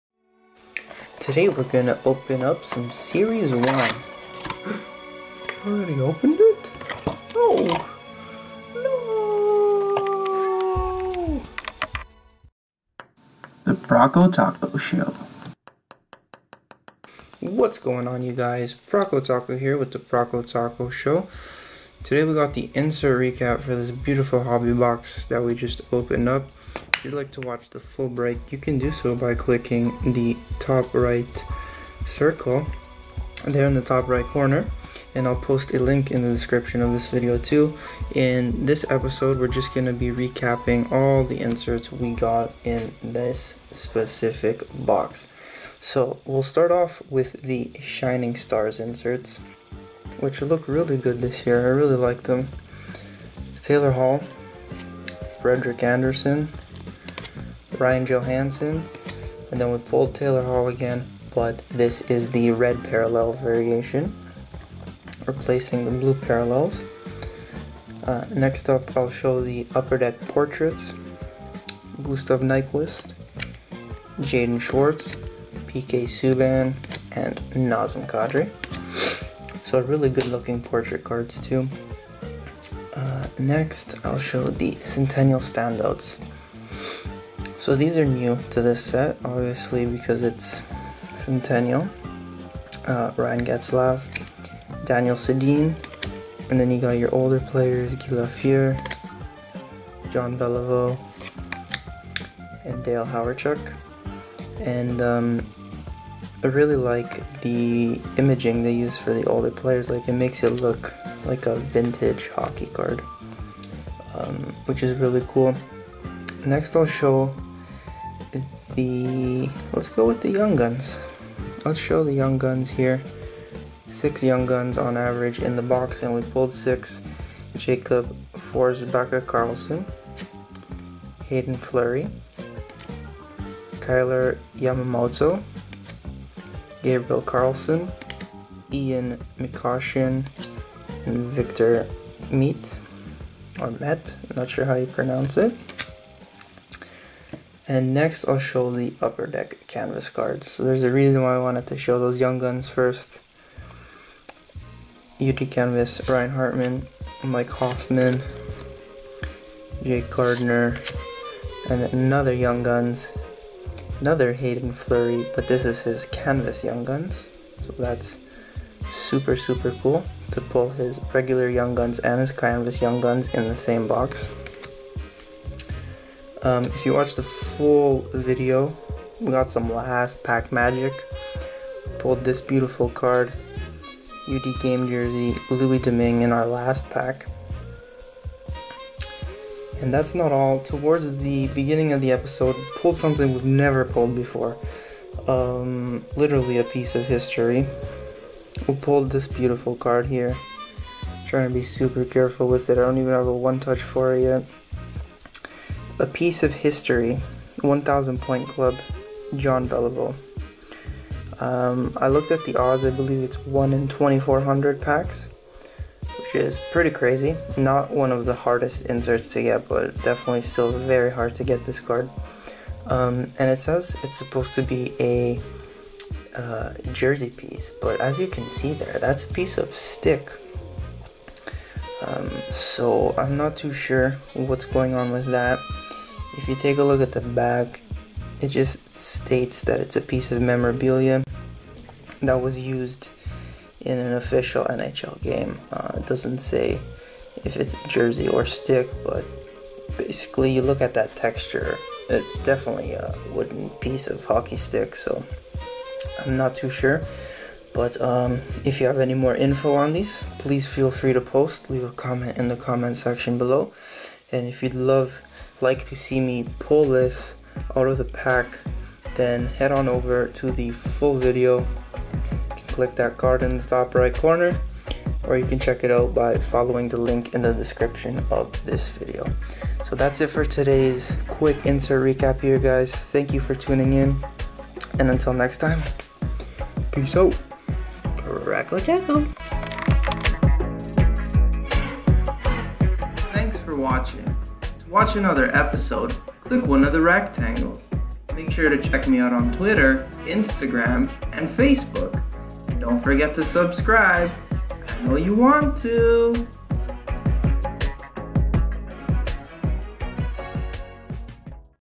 INSERT RECAP - 17-18 SERIES ONE Hobby Box - YOUNG GUNS & 1000 POINTS CLUB!!
What's up Prokotaco Nation!?! In today's episode we recap a hobby box of 17/18 SERIES ONE Hockey cards!

Any PURCHASE through this link (hockey, Yu-Gi-Oh!, and Pokémon cards)

CONNECT with @Prokotaco